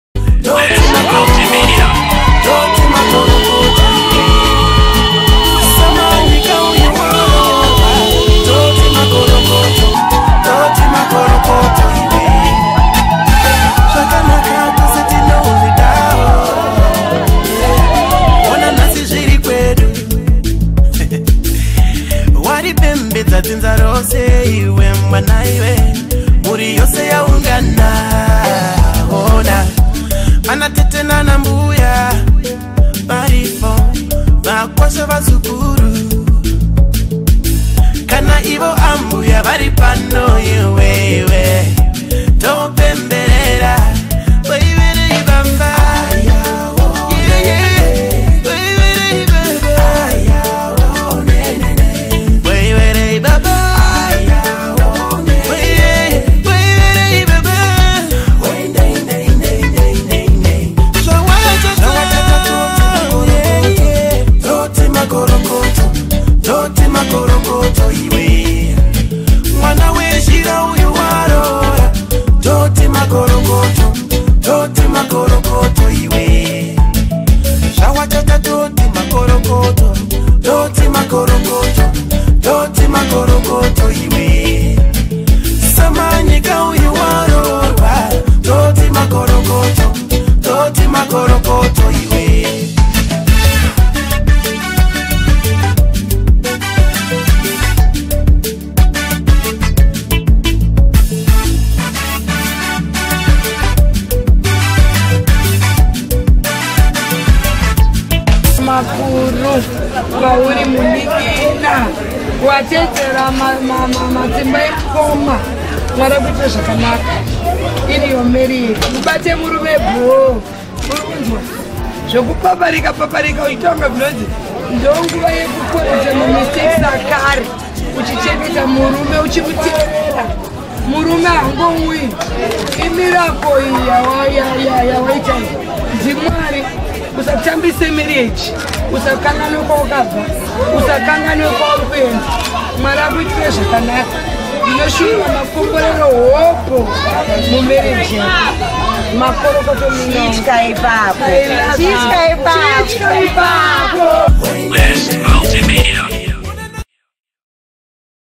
Mai Titi at Lobola Shoot Maggie + Ray, Dancing to Nox Song 15 Dec 2019
15 Dec 2019